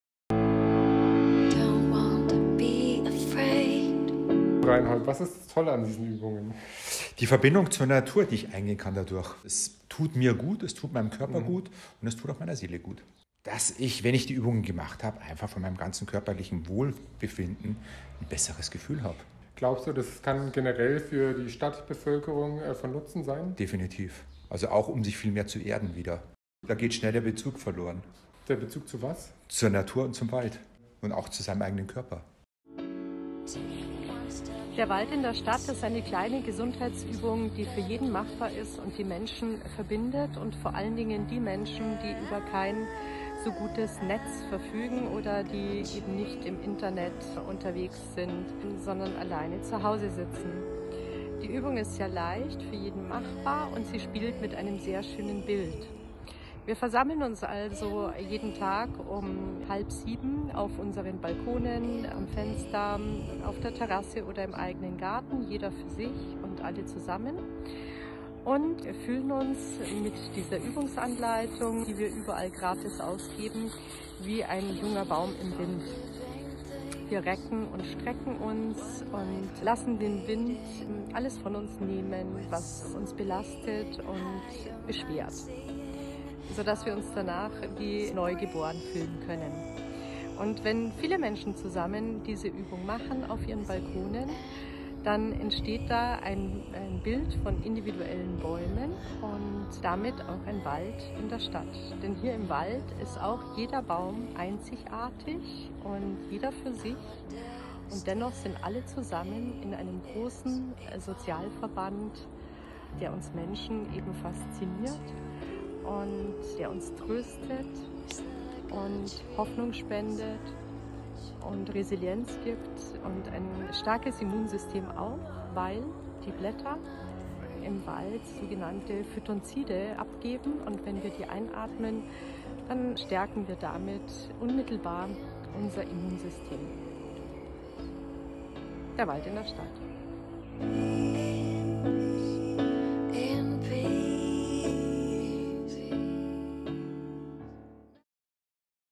Große Mitmach-Aktion "Der Wald in der Stadt". V G3: Gesund & Glücklich mit Vitamin Grün
Sei dabei bei unserer großen Mitmach-Aktion und unterstütze unser Ziel, diejenigen Menschen in unserer Gesellschaft zu erreichen, die über kein (so gutes) Netz verfügen. Für sie haben wir die VITAMIN G3 Gesundheitskarten entwickelt, die man in Apotheken und Geschäften des täglichen Lebens, im Buchhandel und auch bekommen kann. Es geht um Hoffnung, Freude und Gesundheit, in Zeiten, die wir nie für möglich gehalten hätten – ganz elementar und analog.

Mach einfach mit, es kostet dich nichts und bringt viel Spaß! Was du tun kannst, ist ganz einfach und sehr effektiv:
▶︎ Verteile sie ggf. in deiner Nachbarschaft
▶︎ Stell dich abends um halb7 auf den Balkon, ans offene Fenster,  auf die Terrasse, in den Garten
▶︎ Lass zusammen mit deinen Nachbar*innen einen Wald in der Stadt entstehen, um unsere Resilienz und unser Immunsystem zu stärken.
▶︎ Zeige mit Videos, Fotos, Posts, Tweets & Co. unter dem Hashtag VitaminG3gegenCorona der Welt, wie wir in Zeiten von Ausgangs- und Kontaktbeschränkungen Covid-19 trotzen. Jede*r für sich und alle zusammen!
▶︎ Fragst du dich: Warum einen Wald? Ganz einfach: Weil wir ihn lieben! Alle Mitmachgründe, Beweggründe und Hintergründe zu IM-WALD-SEIN auf Balkonien findest du